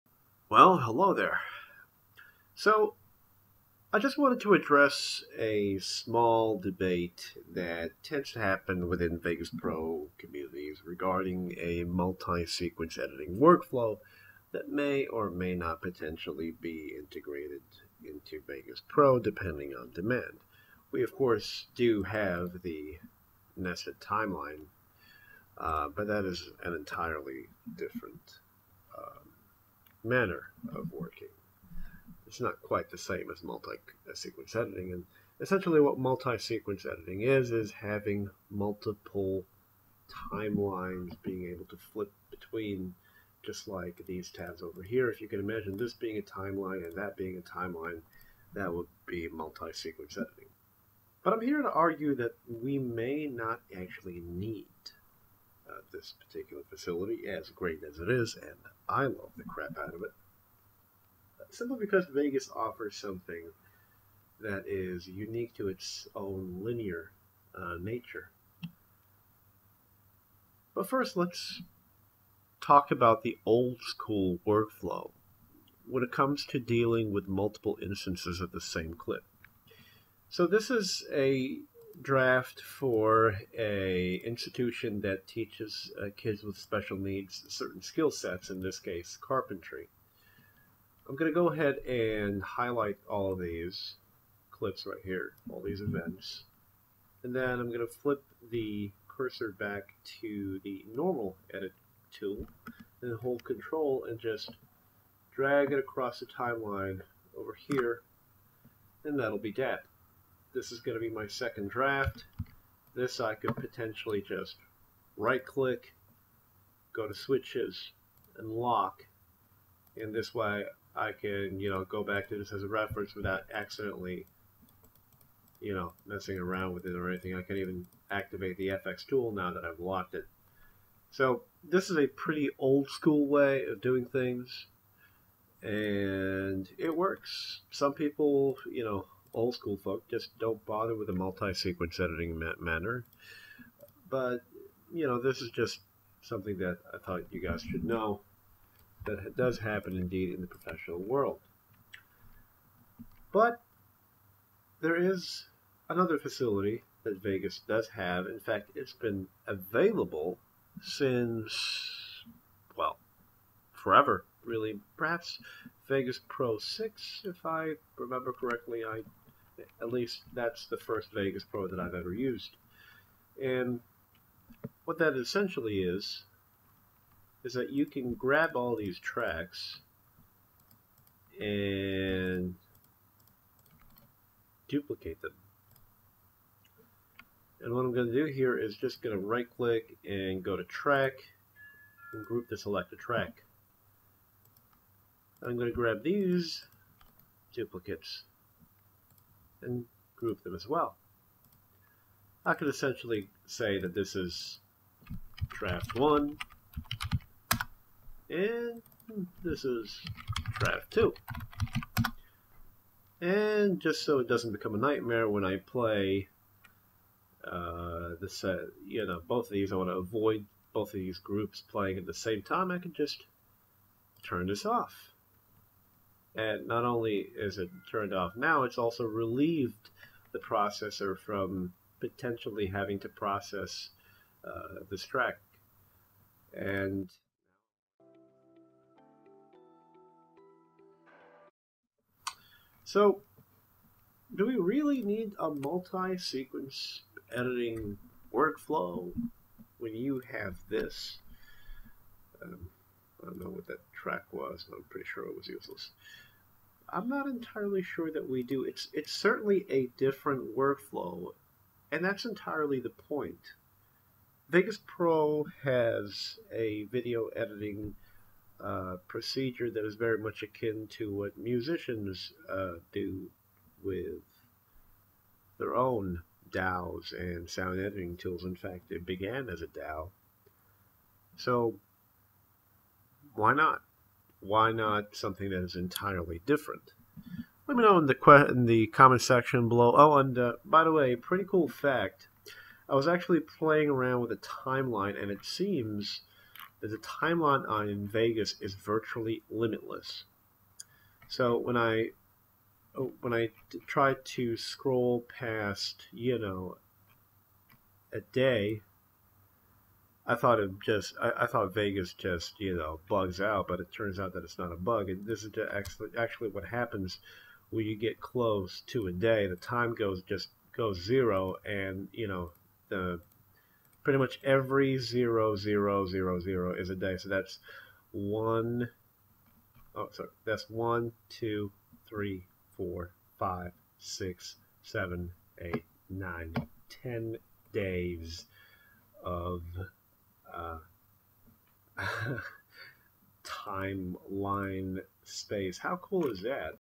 Multi-Tasking in Vegas Pro - A Possible Alternative to Multi-Sequence
What I'm demonstrating here has been around since VEGAS PRO 6, if not earlier., in terms of what the software can do when it comes to multi-tasking.

This may or may not be the answer to users yearning for Multi-Sequence editing to be implemented in VEGAS. While I use Adobe Premiere whenever I work in a production house, I can't objectively say whether or not this is better or slower than working with multiple sequences.

I leave